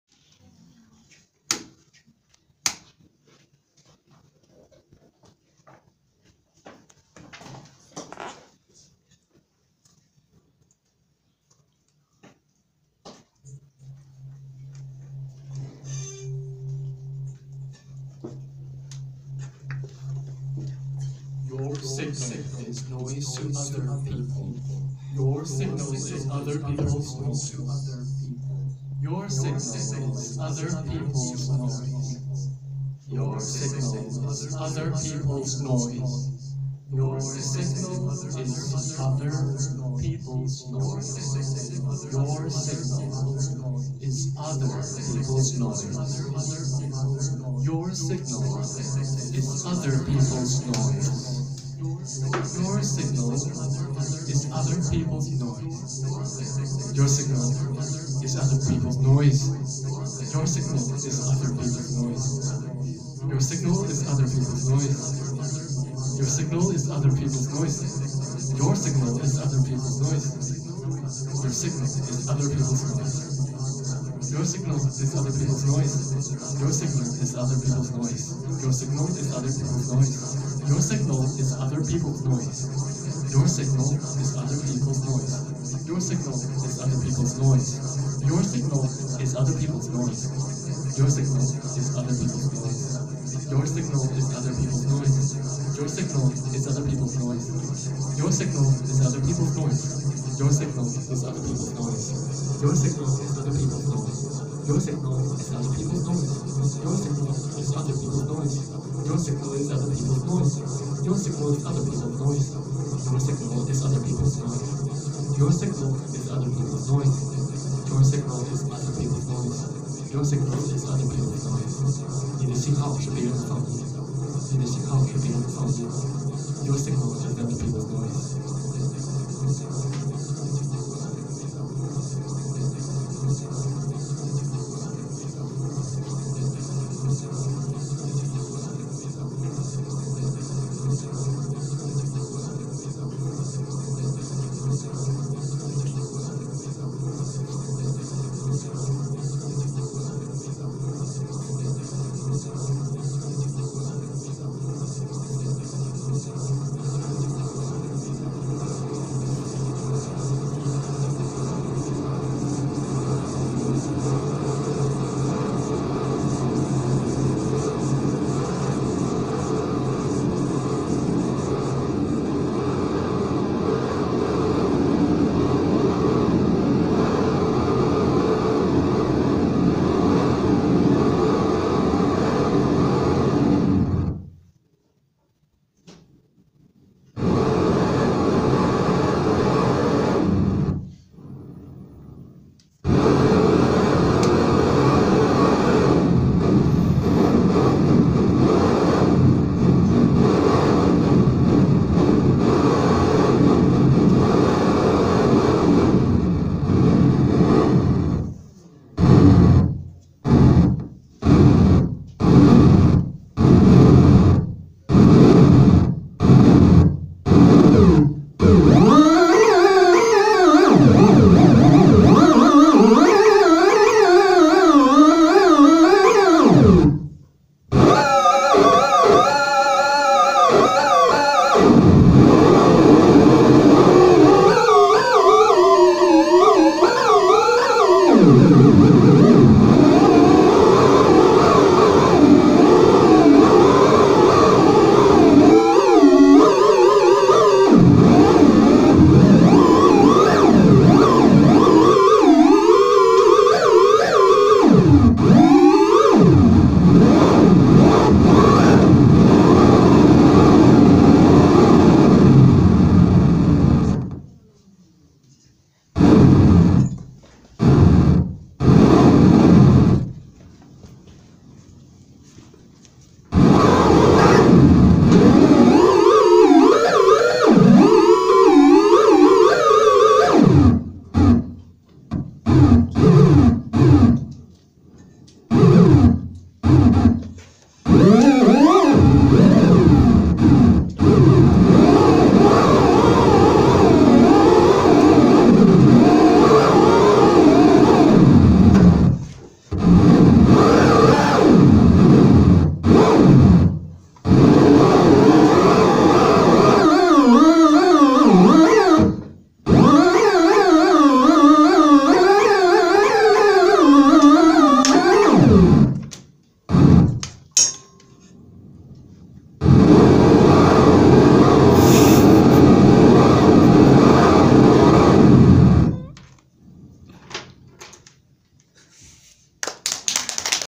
Signal is Noise + Chord Swiper with LEDs
Generate angry signals with analog circuits. Supervised by Eric Parren.